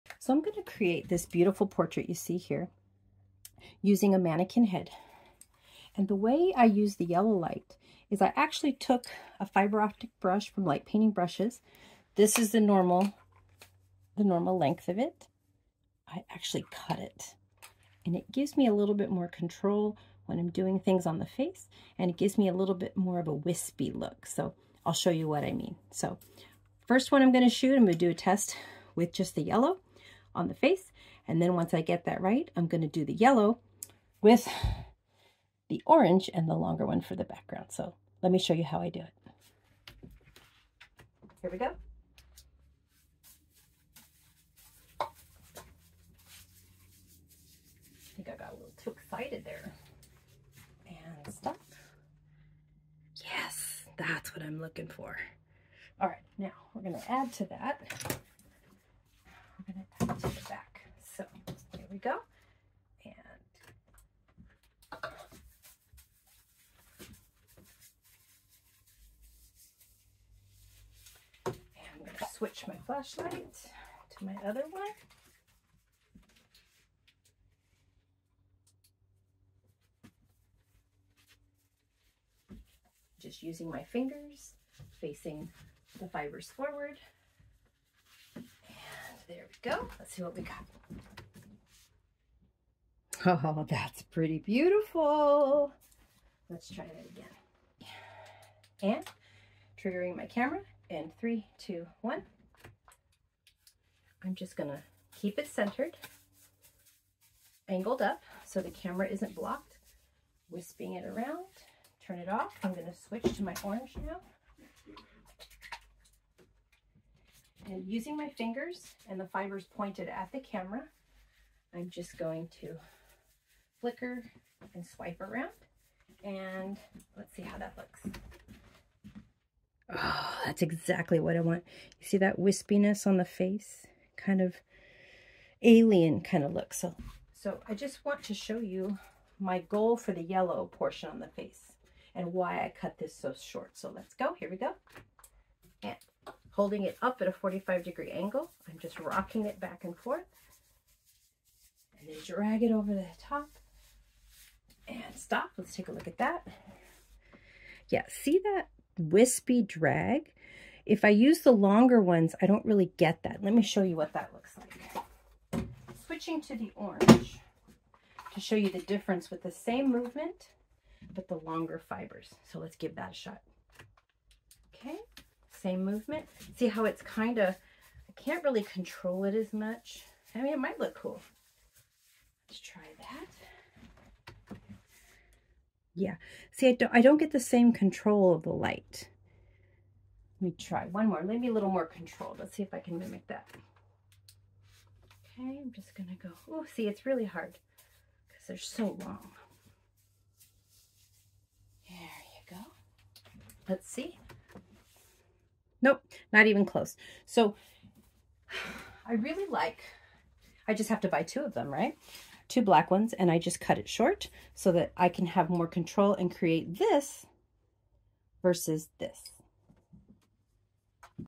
Fiber Optic Light Painting Tool Tutorial
Watch why I cut my fiber optic brush in half to get more control over my light painting image!

f 6.3 BULB mode at ISO 100 using the Canon EOS R5 with a wireless trigger